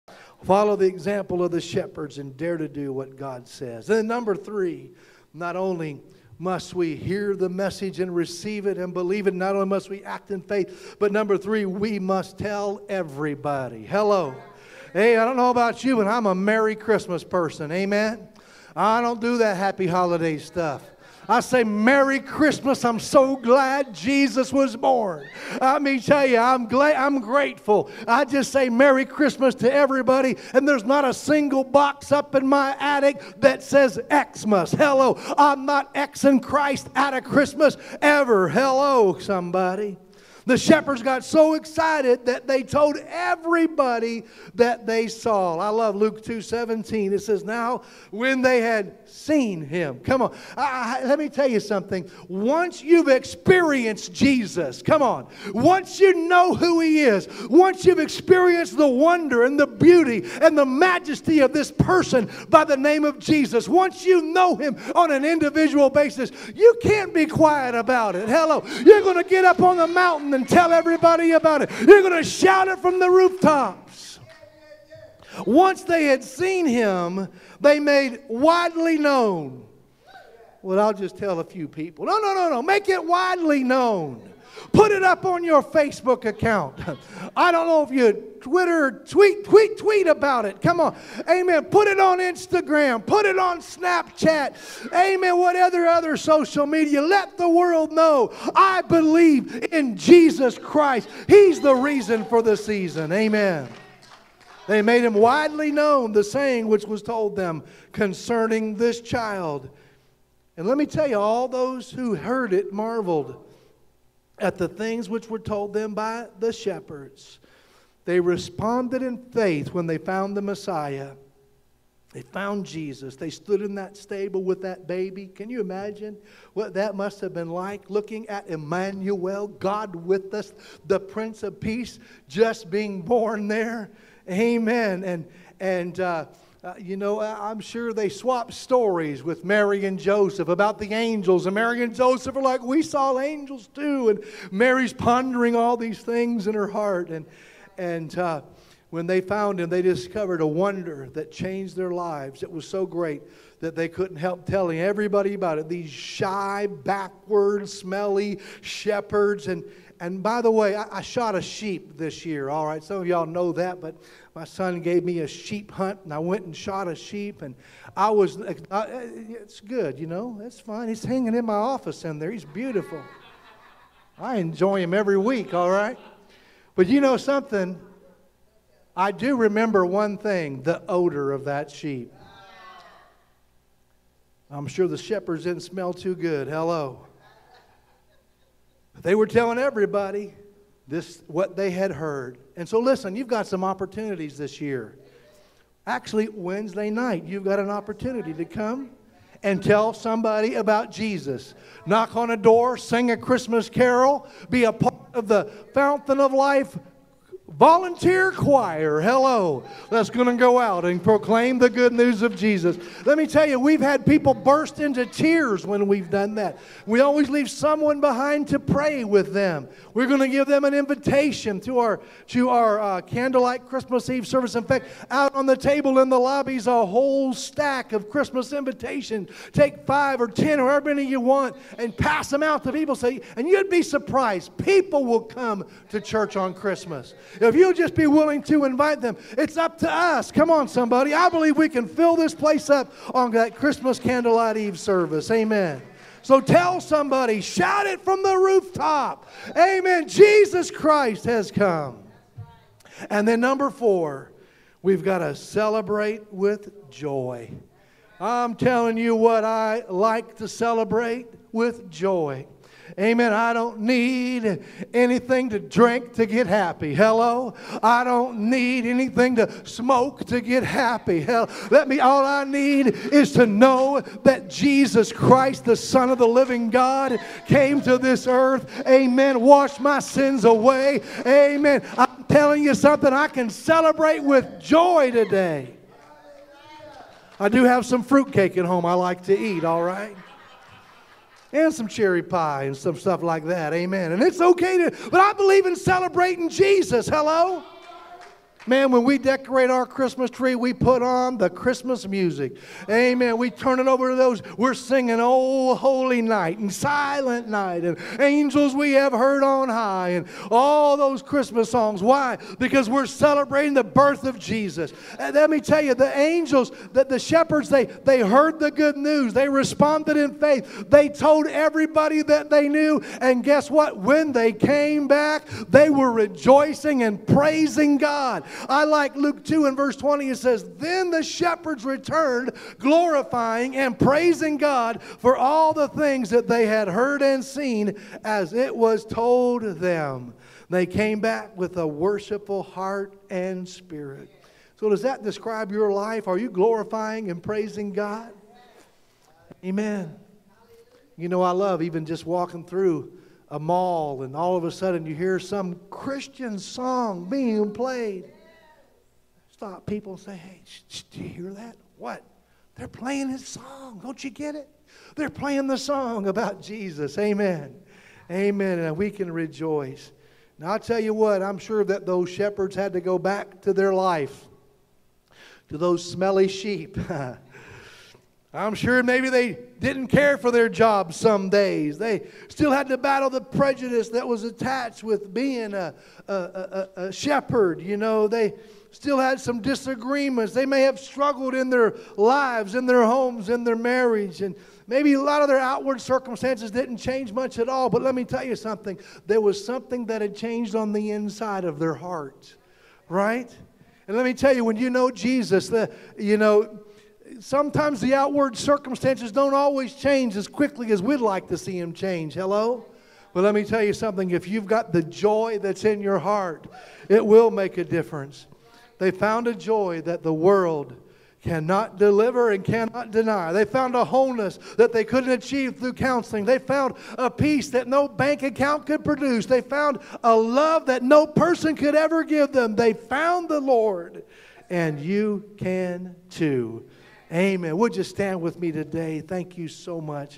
Like the Shepherds: Tell Everyone and Rejoice in Christ
The shepherds are a wonderful example to all how to respond to the good news of great joy that Jesus was born. They heard the news and received it, they acted in faith, and they also told everyone they met so that the good news about Jesus spread.  Once you have met Jesus and have experienced His love and grace you will certainly want to tell everyone you meet.  They also returned praising God and rejoicing.  Will you spend this season focused on the unimportant details or will you praise and worship God from the heart.